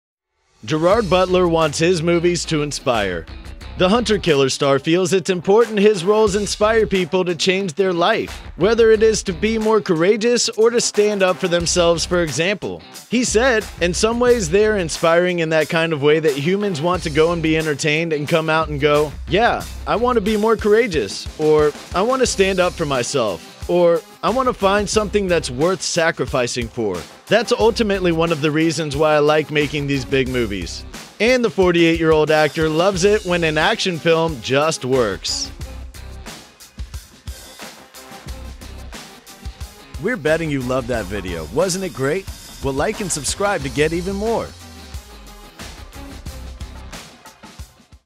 Gerard Butler wants his movies to inspire | Daily Celebrity News | Splash TV
The 'Hunter Killer' star feels it is important his roles inspire people to change their life - whether it is to be more "courageous" or to "stand up for themselves", for example.

(ADD IN TRANSCRIPT)